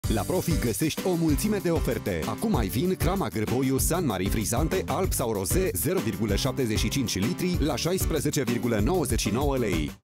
La Profi găsești o mulțime de oferte! Acum ai Vin Crama Gîrboiu St. Marie Frizzante 0.75L la un pret
La Profi găsești o mulțime de oferte! Acum ai Vin Crama Girboiu St. Marie Frizzante alb sau rose 0.75L la un pret special.Oferta supusa unor termene si conditii. Profi. Zilnic preturi mici.